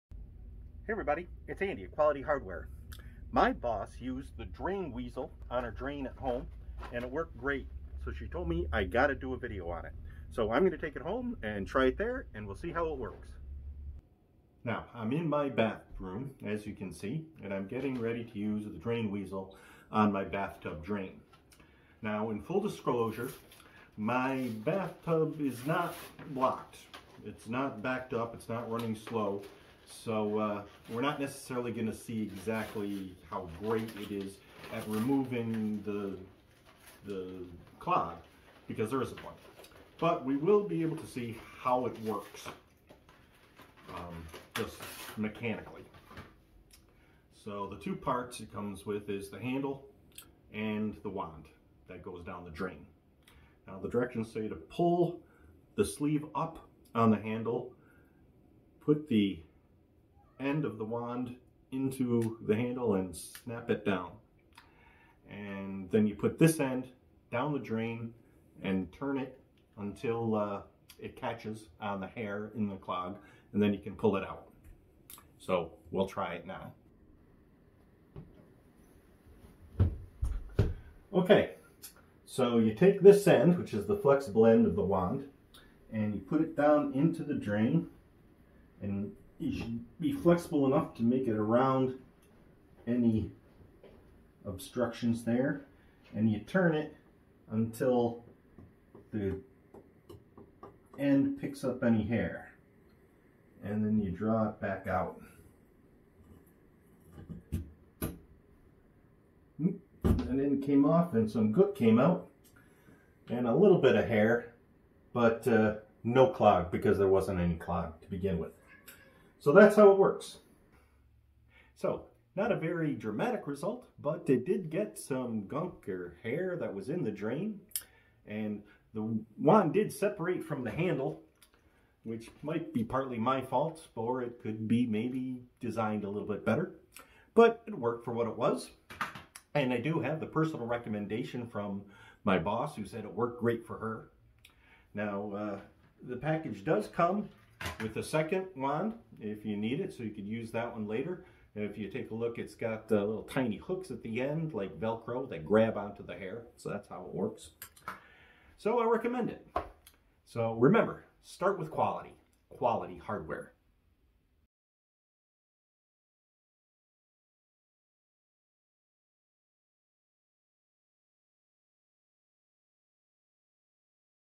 Drain Weasel
I tried the Drain Weasel out on my tub drain.  Take a look to see how it worked.  Start with Quality, Quality Hardware